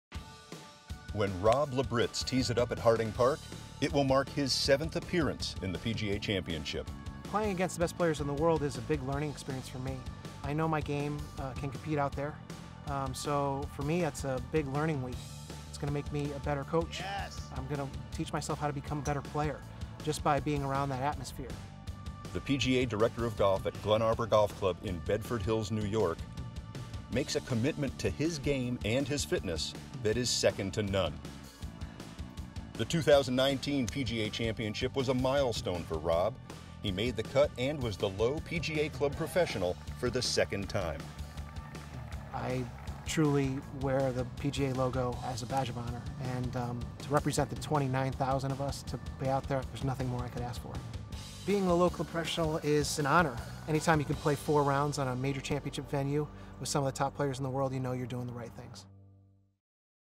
Team of 20 at the 2020 PGA Championship: Rob Labritz
Rob Labritz is focusing on learning this week at the 2020 PGA Championship to help him to be a better coach and player. He is playing in his seventh PGA Championship.